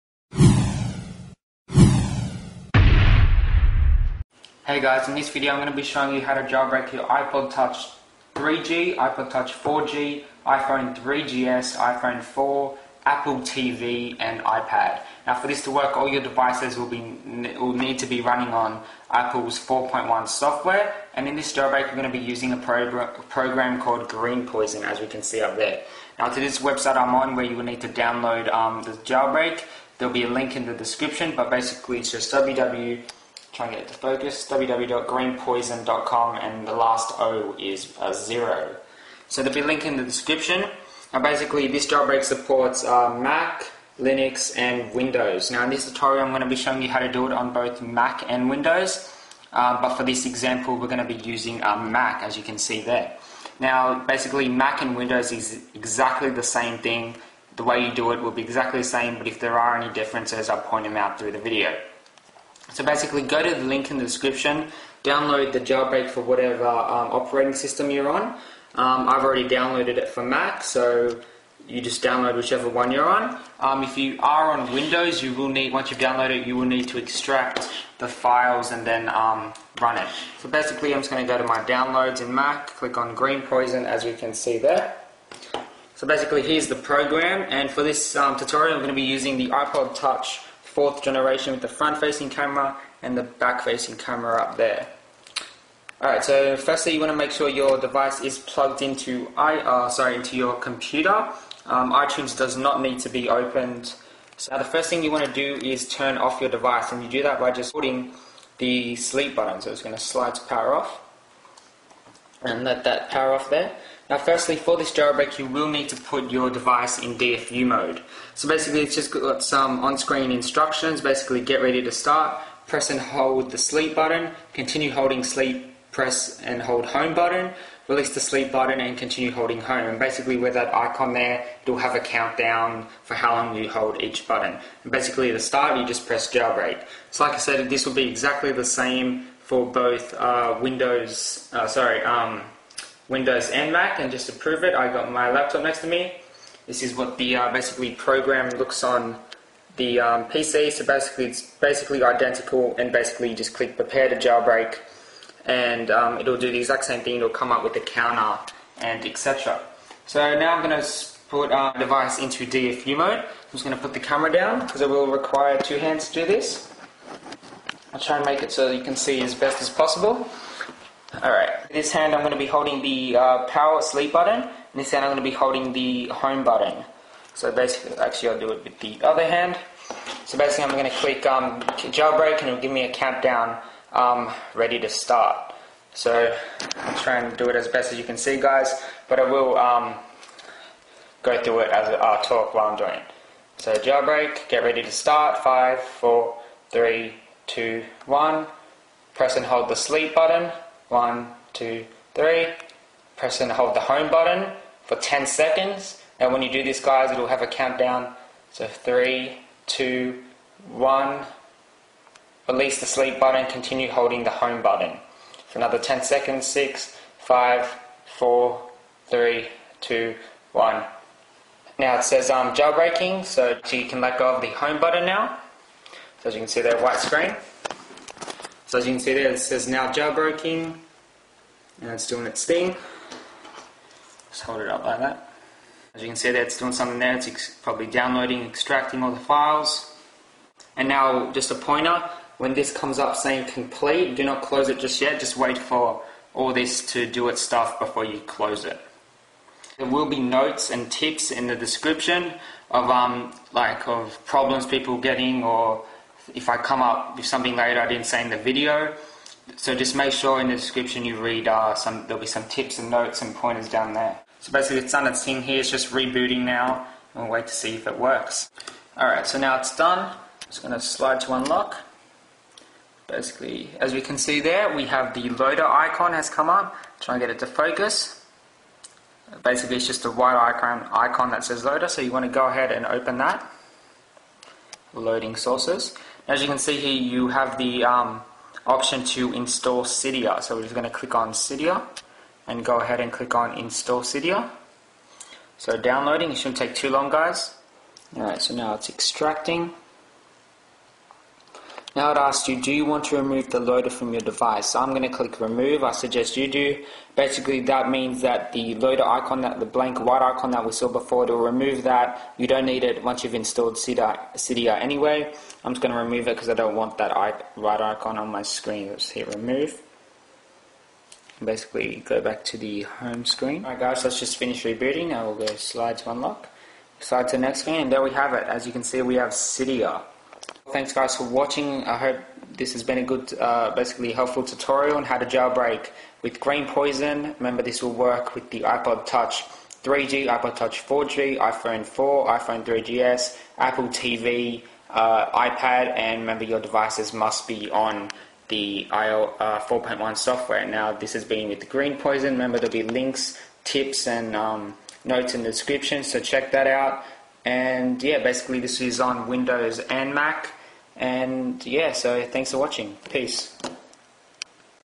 Jailbreak 4.1 iPhone 4, 3GS, iPod Touch 4G, 3G, iPad on Mac w/ Greenpois0n [HD]
This Tutorial whill show you how to jailbreak your iPhone 4, iPhone 3Gs, iPod Touch 4G, iPod Touch 2G on iOS4.1 with GreenPois0n on Mac and Windows...

Notes:
- Jailbreaks iPhone 3GS, iPod Touch 3G, iPad, iPhone 4, iPod Touch 4G
- Works on firmwares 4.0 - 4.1 + 3.2.2 on iPad
- The iPhone 3GS new bootrom bug has been fixed.
- AppleTV is technically supported, but theres no apps yet
- You may need to restart to get the Cydia icon.
- If you have problems, remember this is buggy. Try it a few times or wait for a newer release.
- I had some issues at first but I updated to the latest version of iTunes and it fixed them.
- iPod Touch 2G support coming soon
- For iPhone 3G, use redsn0w (i have videos for mac and pc)

If you have any problems leave a coment below

Thanks
MadAssTutorialz